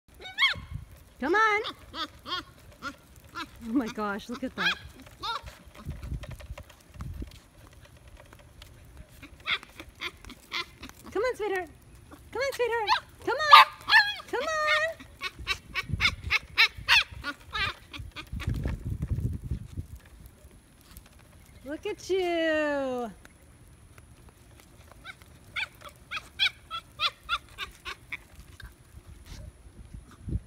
DYI temporary Quad Cart
This Chow mix puppy was growing too fast to put into a cart. He is blind with other neurological issues. The foster mom built this using a baby stroller. A stronger cart ( if needed) will be built when he is more fully grown. 1/2 of the dogs with carts rehab to not needing them.